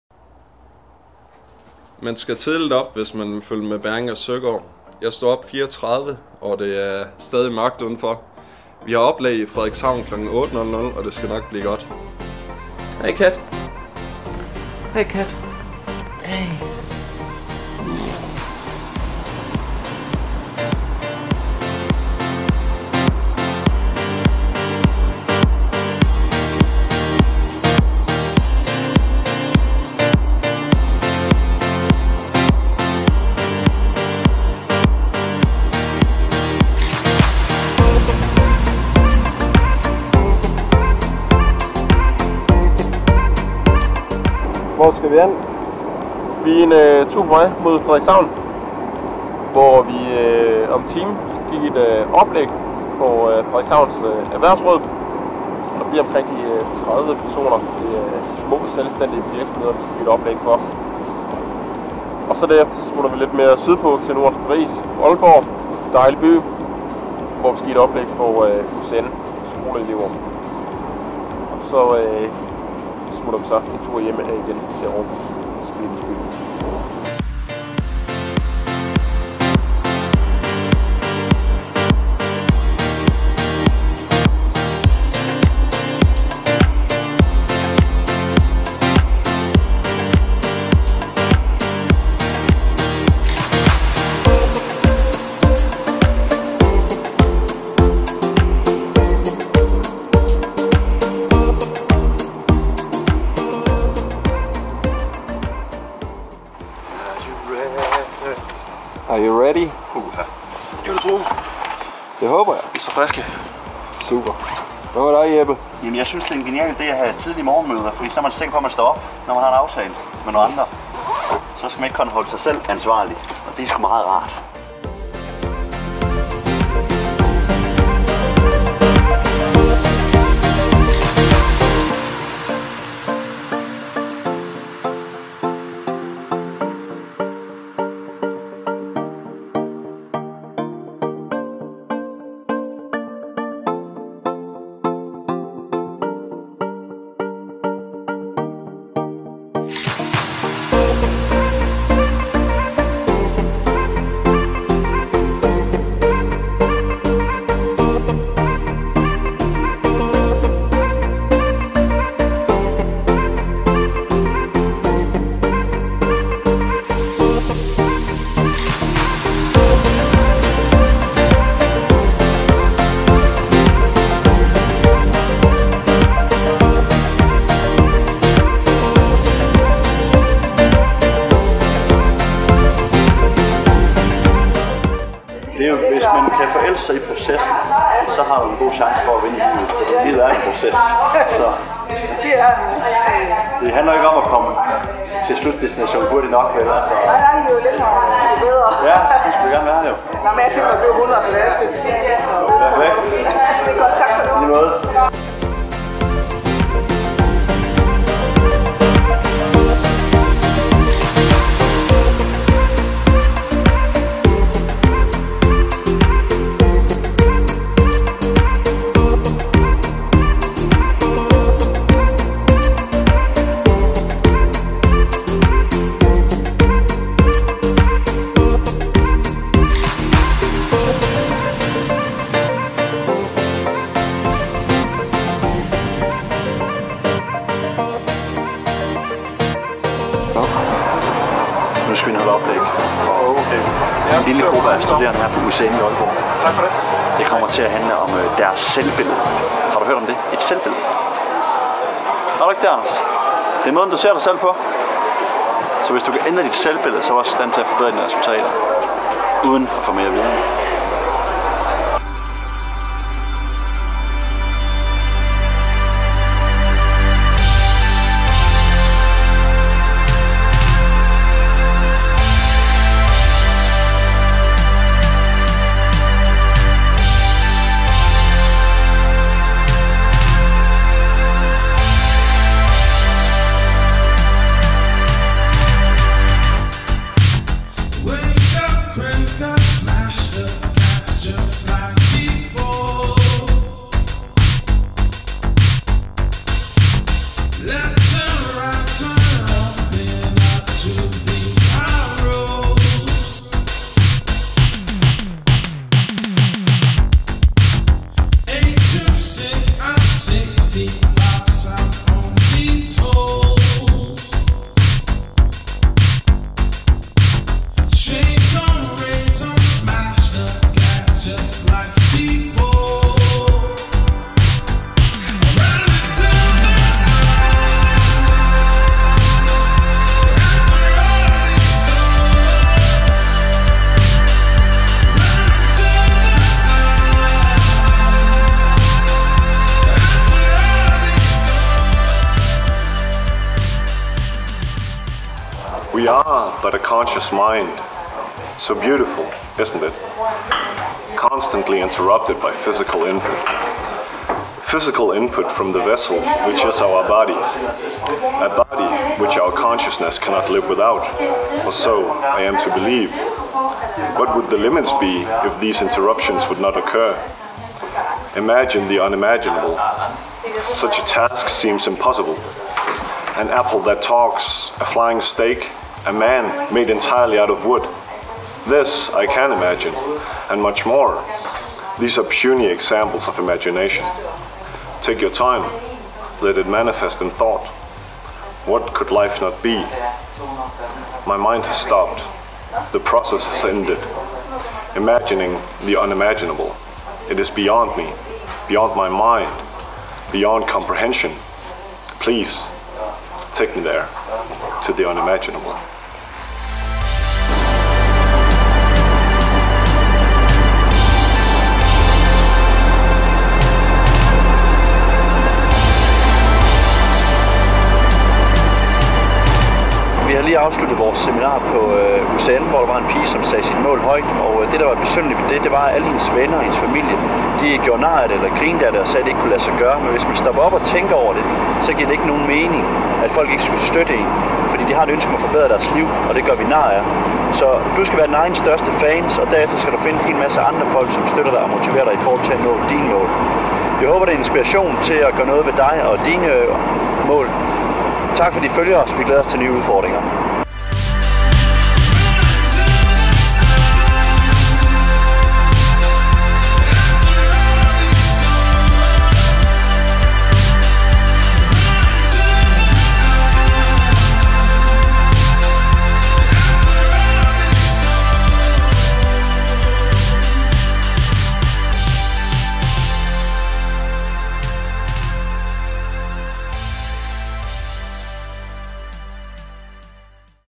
FREDERIKSHAVN OG AALBORG PÅ SAMME DAG | VLOG 3
Kasper, Jeppe, Anders og Lukas rejse fortsætter i starten af næste uge, så kom ind og kig med.

Tjek vores hjemmeside ud for mere inspiration til et endnu gladere, sundere og mere produktivt liv